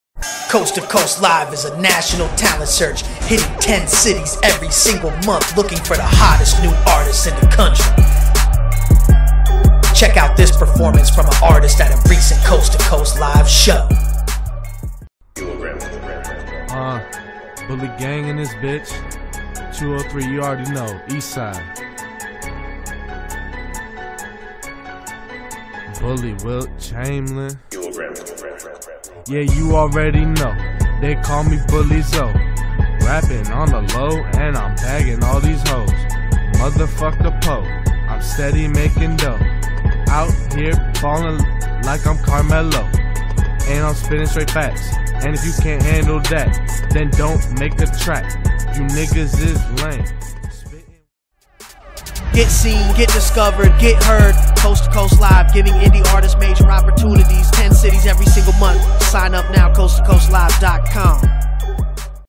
Bully Chamberlain (@bullyzo) Performs at Coast 2 Coast LIVE | Connecticut All Ages Edition 7/25/16
DJ Jack Frost - Ex on air DJ at jamn 94.5 fm , world famous club DJ
DJ Ron G - Legendary DJ/Platinum Producer for J-Lo, 2Pac, Fat Joe, R. Kelly & More
Dj Meechie - Dj / Producer - On Air DJ for Hot 93.7 FM

Monday, July 25, 2016 9:00 PM

The Space
295 Treadwell Street